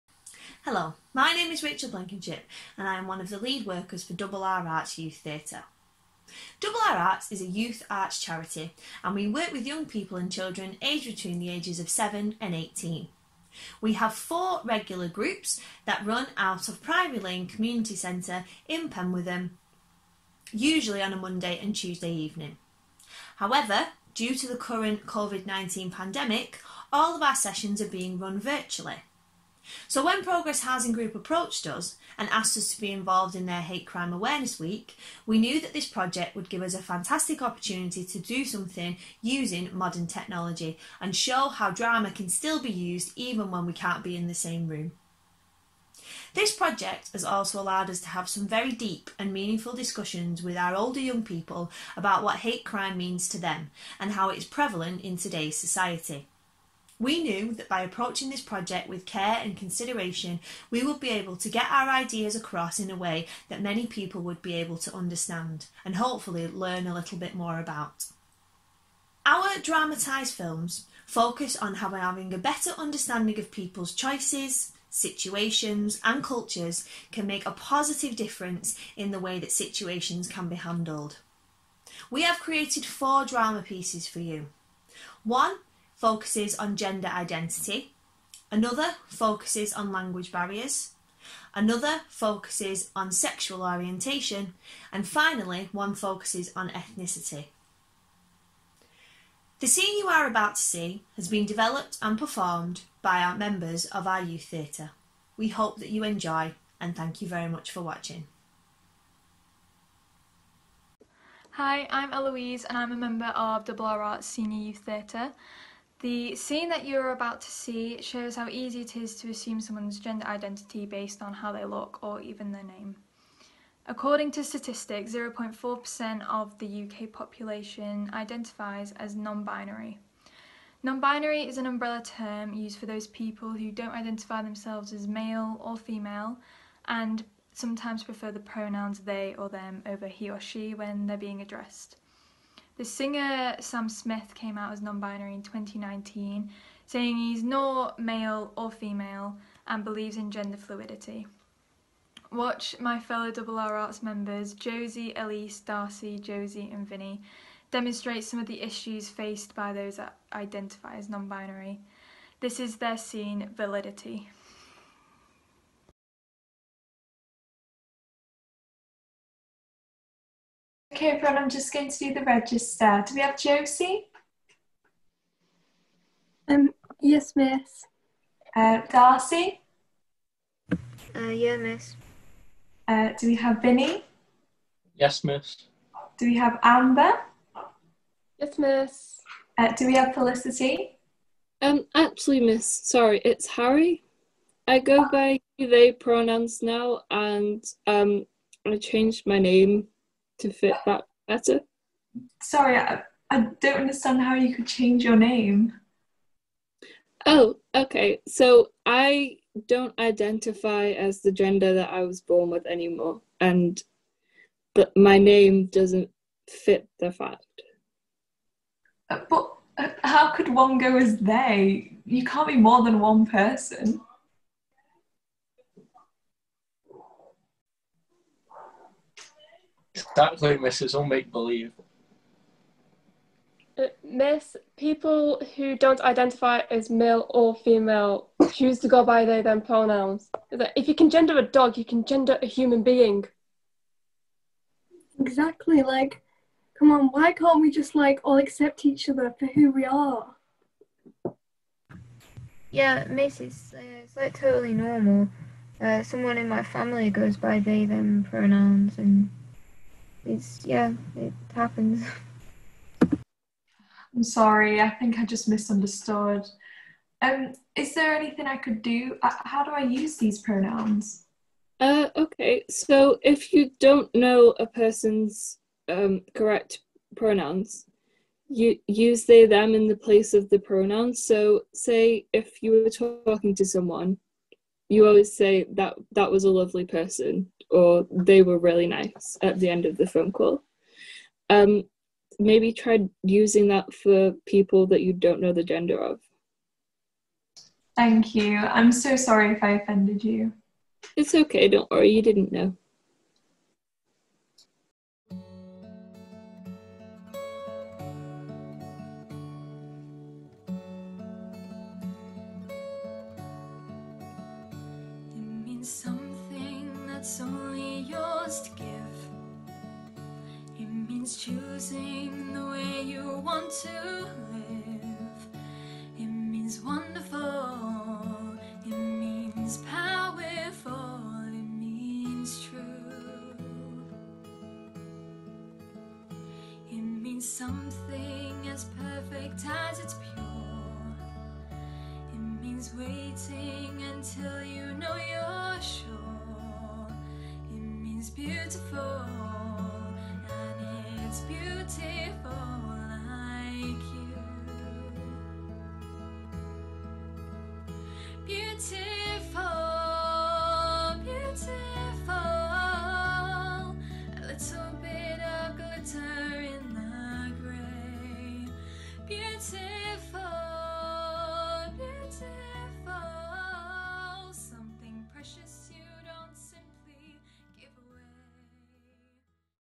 Progress Housing Group - Hate Crime - Validity
We have been working with the 'Double R Arts' Youth Arts Group from Penwortham, who have produced four videos about what Hate Crime means to them. They hope the videos are informative and show that having an understanding of people's choices, situations, and cultures can help grow tolerance.

This video is part of a series for Hate Crime Awareness Week 2020.